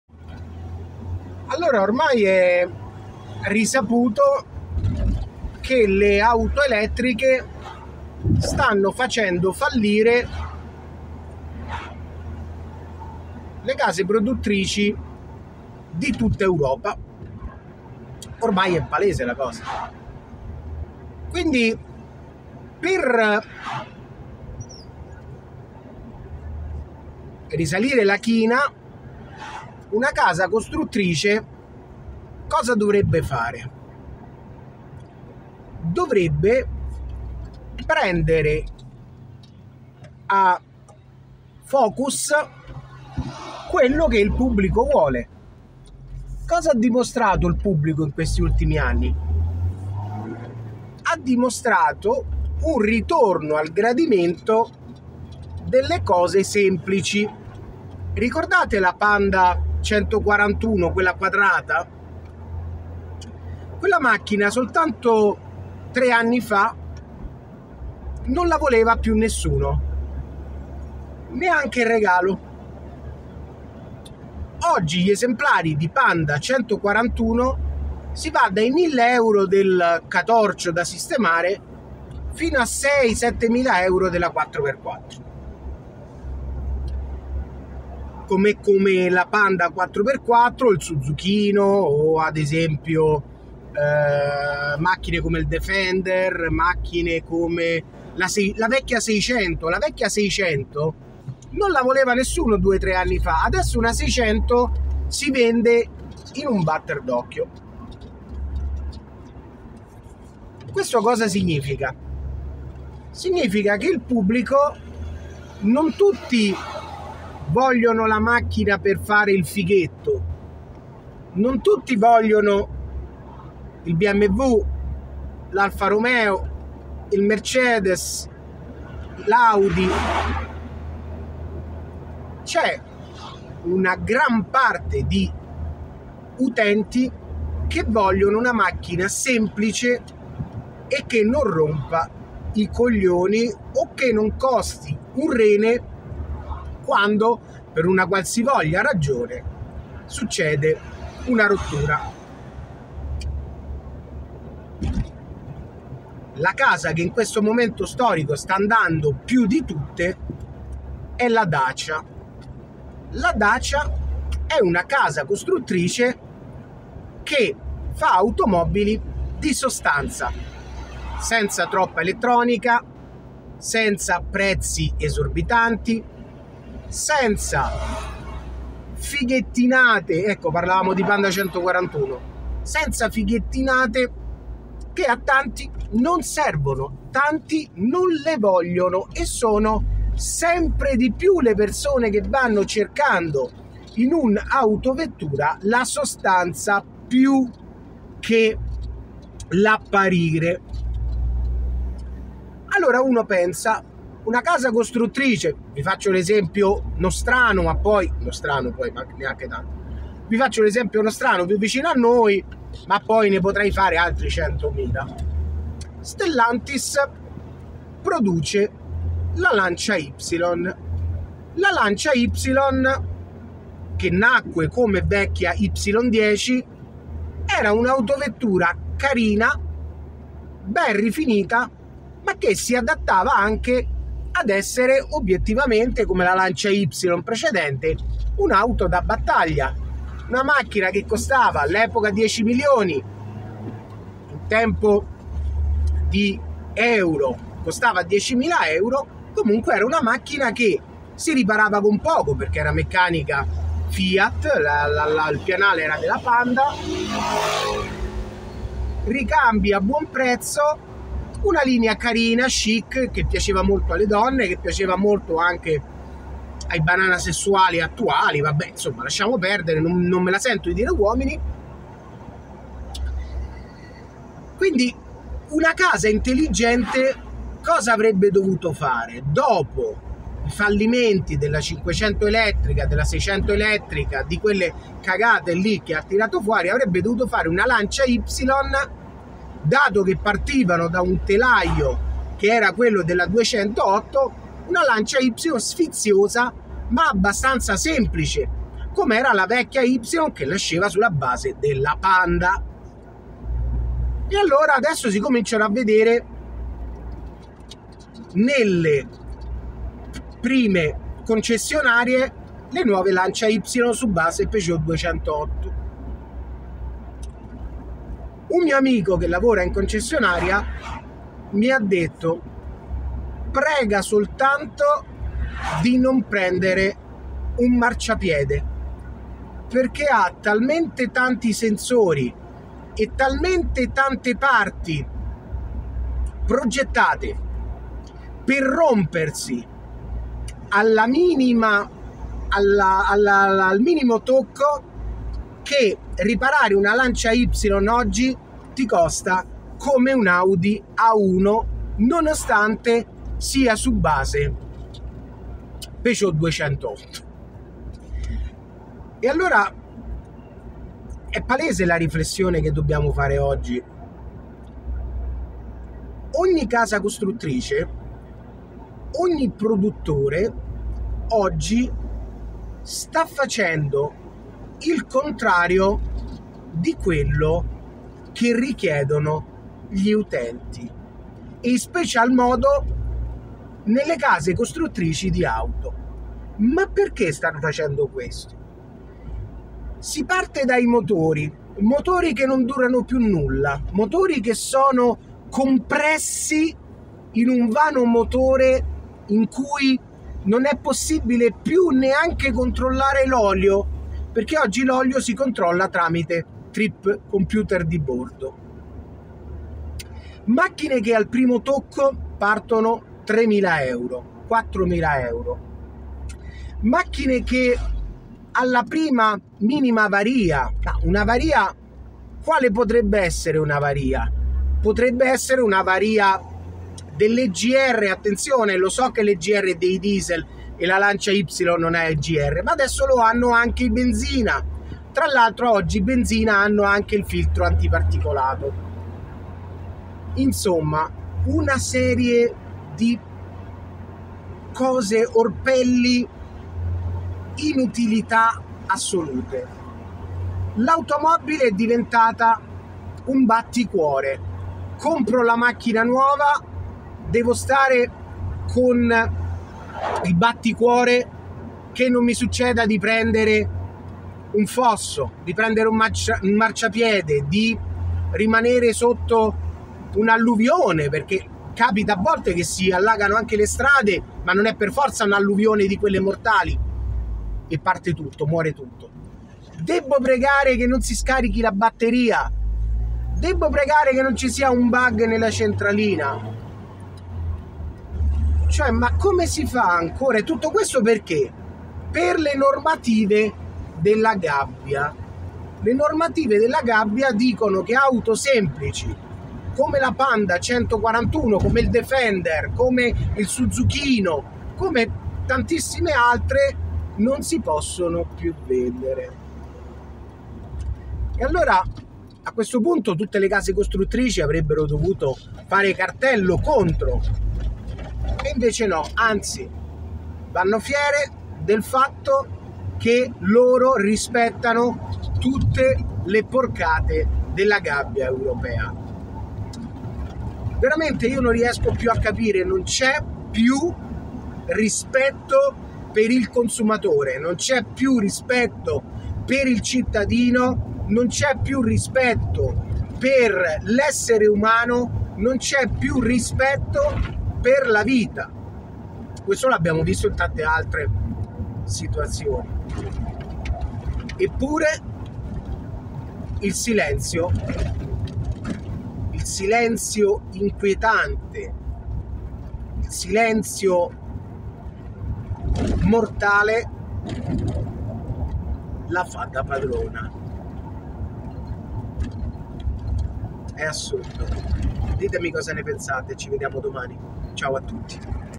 DELL'OPINIONE E DELLE NECESSITÀ DELLA PERSONA NON IMPORTA PIÙ NULLA A NESSUNO‼️ LANCIA YPSILON DOCET
Fino a pochi anni fa produttori di auto e produttori di beni di consumo, ascoltavano le necessità del consumatore, oggi se ne importano totalmente ‼️